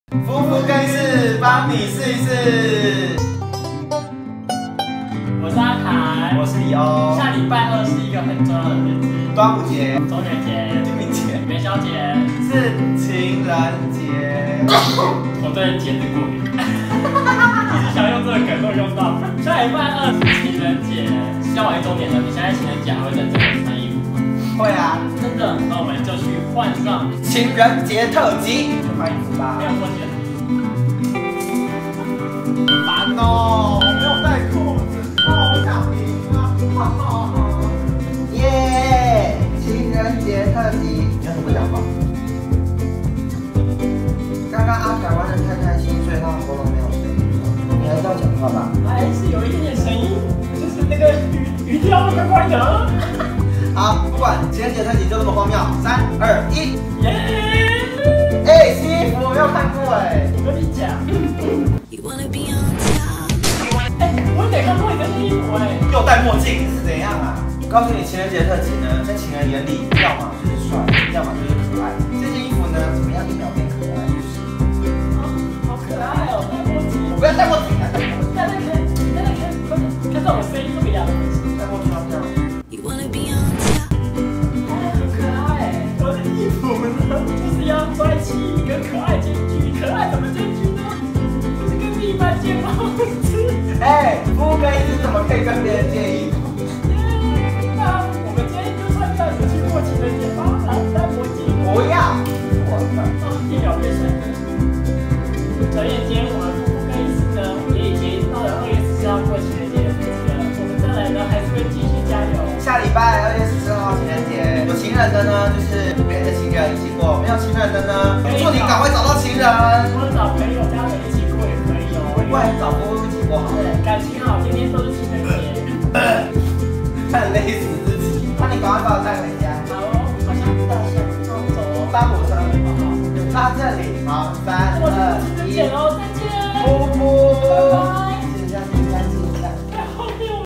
情人節穿搭大PK｜夫夫更衣室Fufushow
老公快點帶我去吃大餐，我要吃好多好吃的東西！（里歐）

一年一度的情人節又要來了！單身的朋友沒有關係，讓夫夫陪你們過吧！(阿凱)